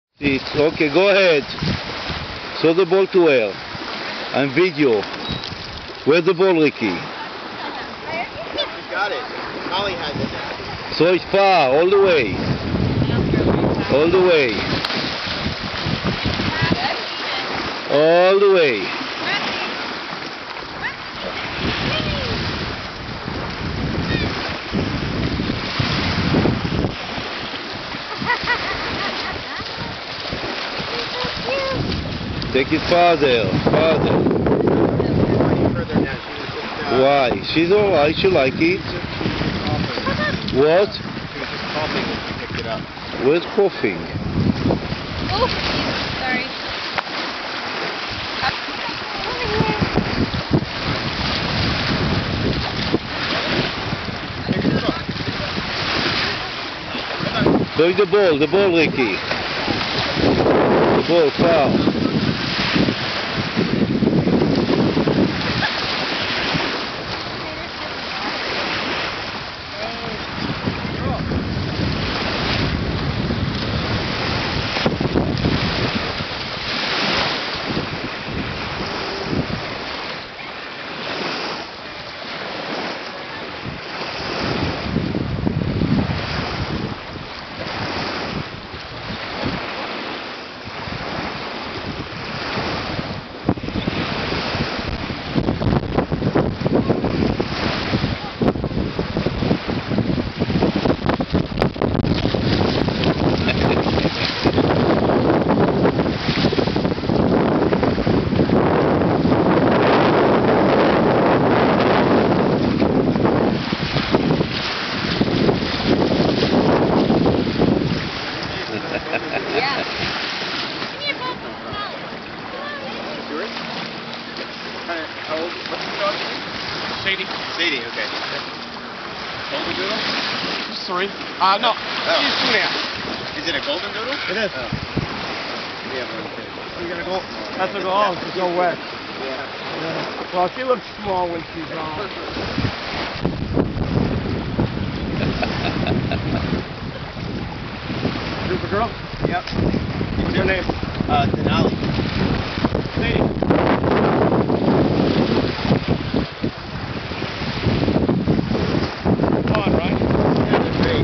Denali and Family in Evanston
Denali plays at the Evanston Dog Park